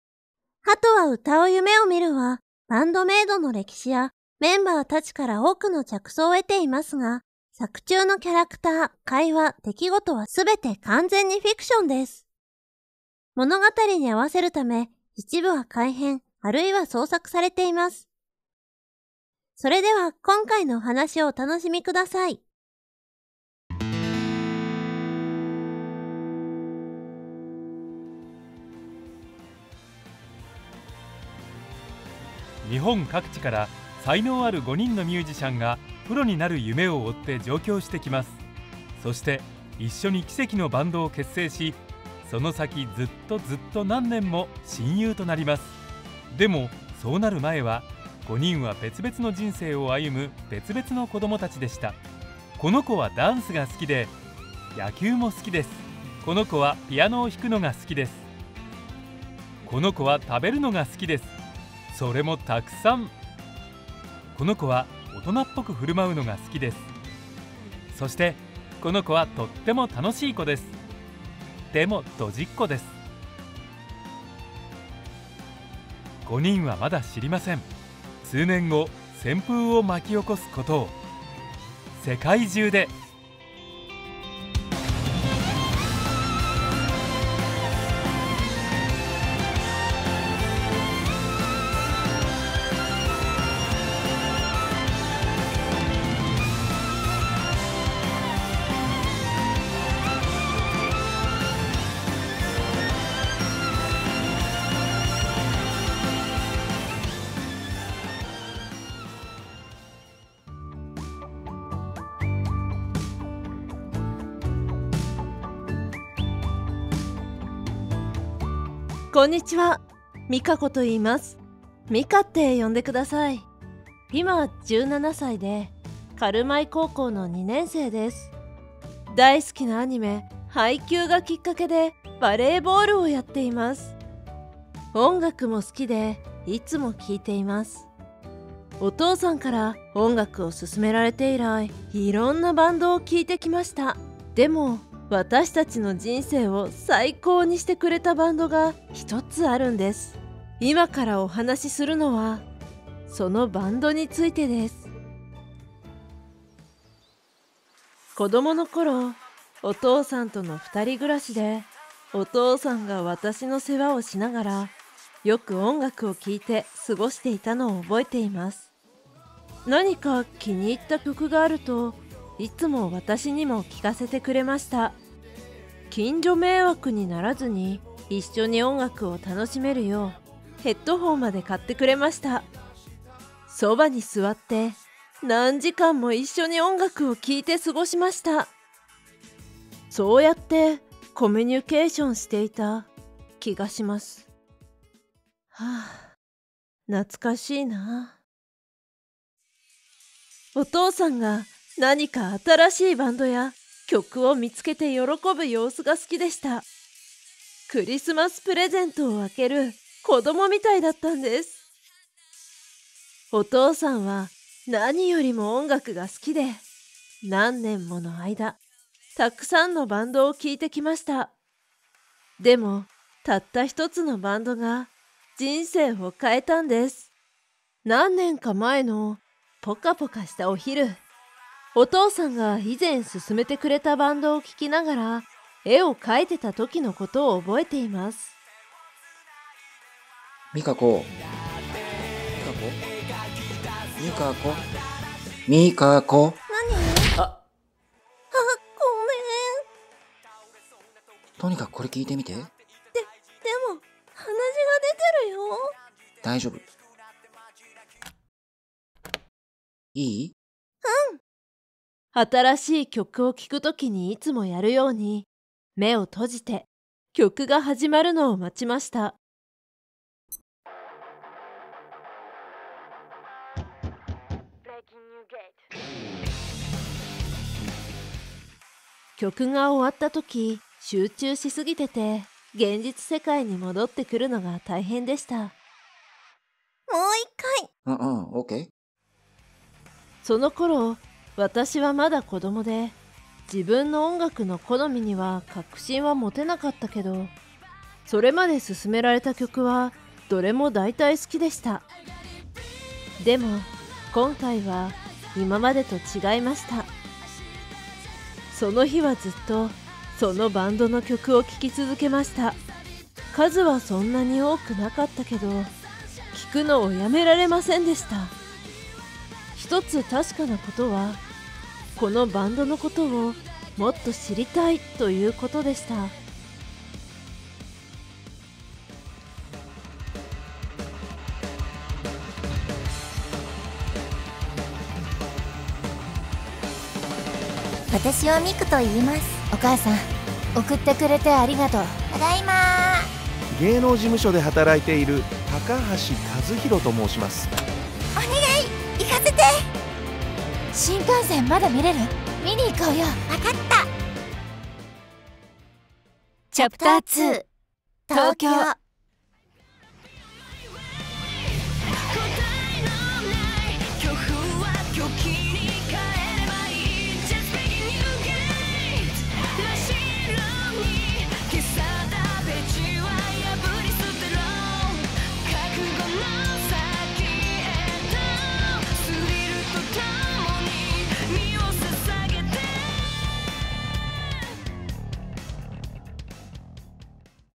BAND-MAID Manga / Chapter 1: Memories 「ハトは歌う夢を見る」／ "The pigeon who dreams of singing"
"The pigeon who dreams of singing" is a fan made manga with voiced characters inspired by the history of BAND-MAID and its members.
All the characters, conversations and events are purely fictional.
Some elements have been altered or created to fit the story.

In this first chapter, we are following Mika and her dad while they discover a new Japanese rock band.

※ Characters
//.Mika - VA: Ayano Irizuki (@Mochijapa)
Mika is a student who loves volleyball and music.
Her father introduced her to a lot of band since she was a child.
He is the reason why she started to listen to BAND-MAID.

//.Mika's father - VA: @t-shinji
Mika's father loves listening to music and discover bands more than anything.
He loves to share his last findings with his daughter.
He raised Mika alone since her childhood.

//.Narrator - VA: Hishida Moriyuki

Video edition & production - @Ohrenje
Script - @Ohrenje
Script check - Kasper Michaels, Simeon and @t-shinji
Script translation (Japanese) - @t-shinji
Casting - @Ohrenje & @Mochijapa
French subtitles - @Ohrenje
Russian subtitles - exDemonius
Belarussian subtitles - exDemonius
Hungarian subtitles - Tibor Barta
Spanish subtitles - rocotocloc